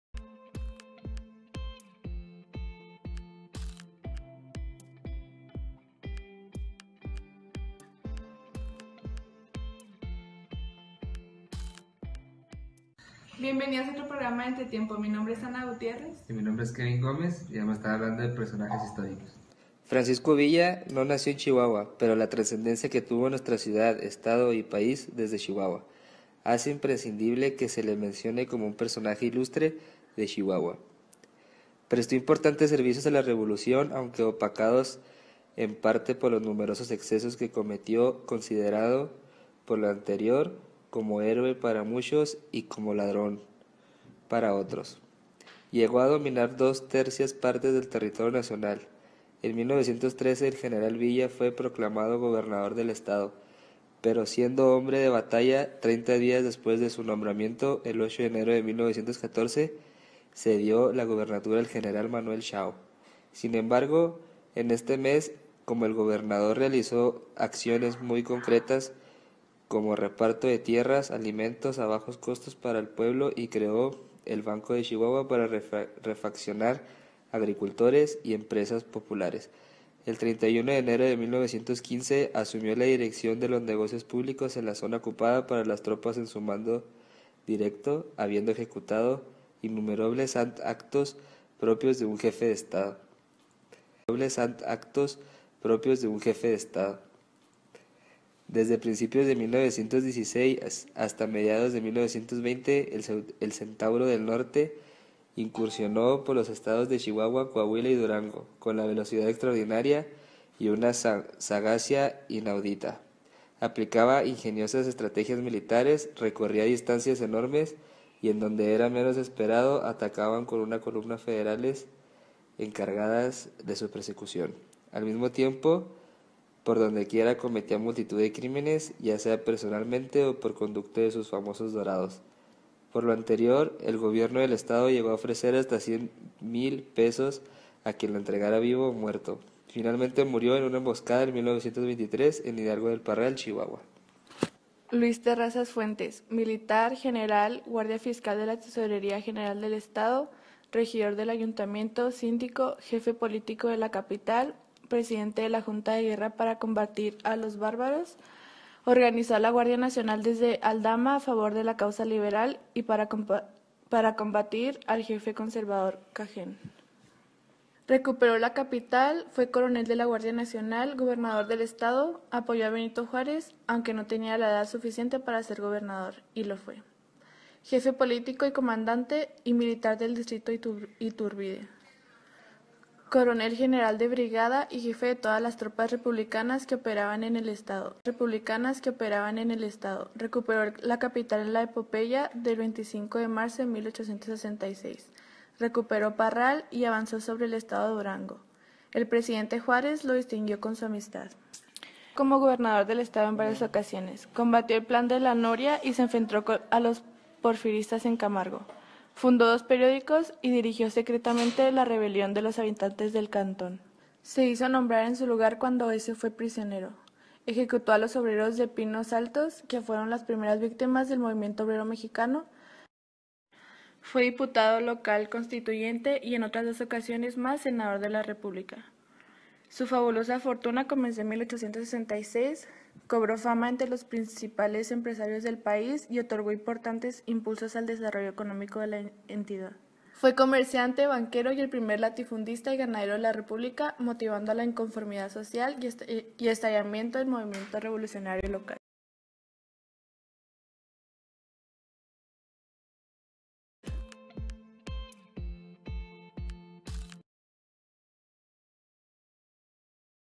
Personajes históricos. Pancho VIlla con Ana Gutiérrezy Kevin Gómez
Trabajo realizado por Ana Gutiérrez y Kevin Gómez de la Licenciatura en Periodismo de la Universidad Autónoma de Chihuahua.